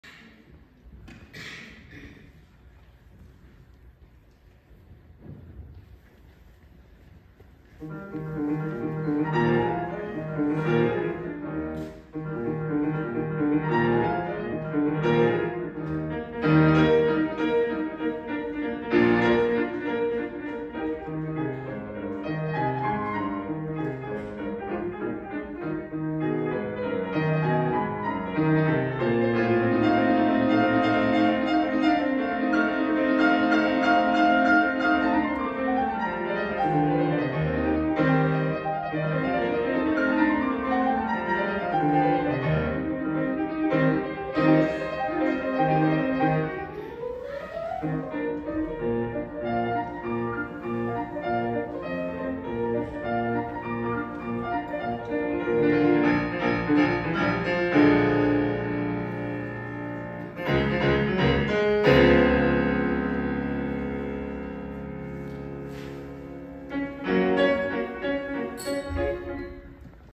[현대 아트 콩쿨]-현대아트대상
울 연준이의 코오오오옹쿨 !!
유튜브에 많은 영상을 남긴 연준이의 콩쿨~~!ㅎㅎ
연준이도 콩쿨전부터 컨디션이 안좋아서 증말 걱정이 많았었눈데….!!
당일날 컨디션 회복 성공 -!!👍💓
울 연준이도 5학년 난이도 높이는데 한 몫 한듯..?😝
우리(연준+슨생님) 항상 겸손한 태도를 가지고 열심히 하자….!🥰
남은 콩쿨도 뽜이팅 ~~!💕